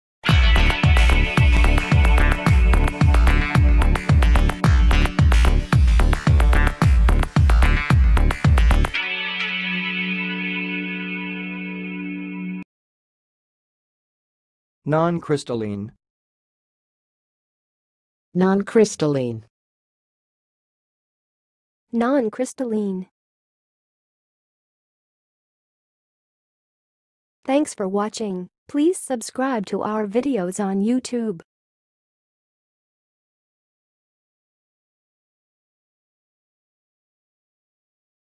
How to say "non-crystalline"! (High Quality Voices)
Watch in this video how to say and pronounce "non-crystalline"!